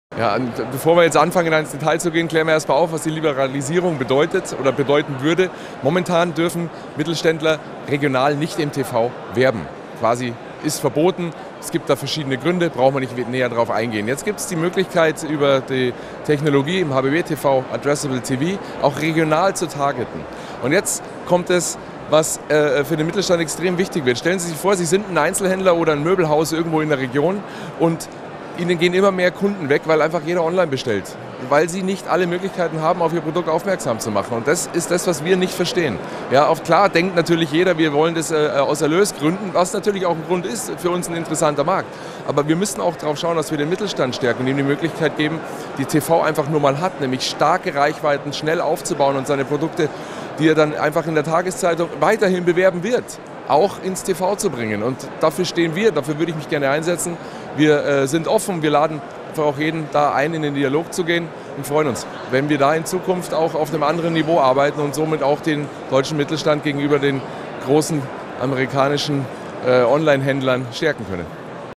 Screenforce 2018: Andreas Kösling, El Cartel Media
Warum der TV-Markt liberalisiert werden muss und regionaler Mittelstand ins Fernsehen sollte.